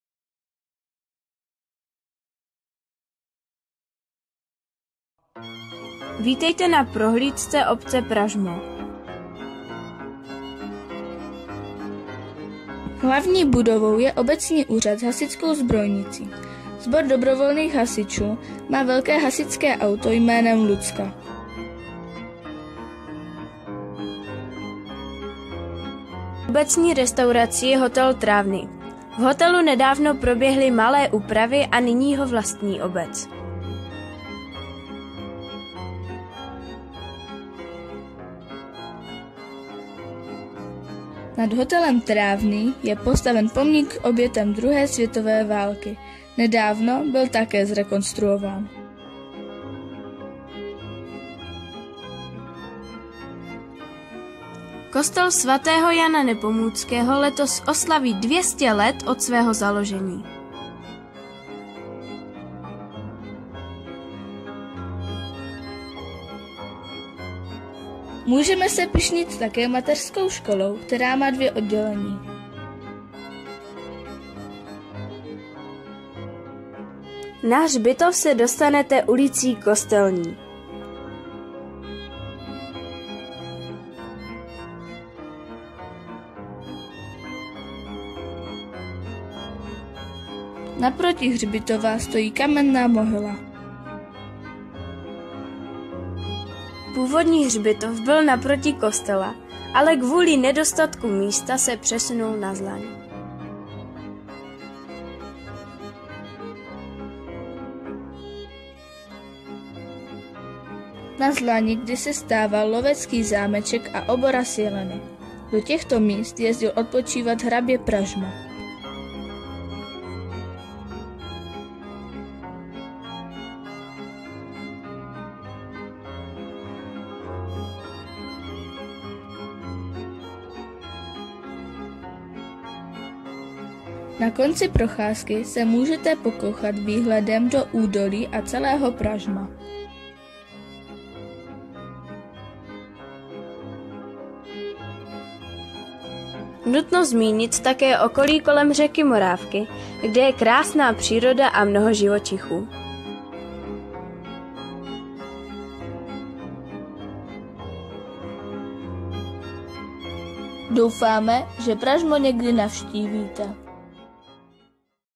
PRAŽMO - ZŠ PRAŽMO KKSB
Soutěžní příspěvek žáků v rámci soutěže prezentací obcí RSB "Křížem krážem Slezskou branou".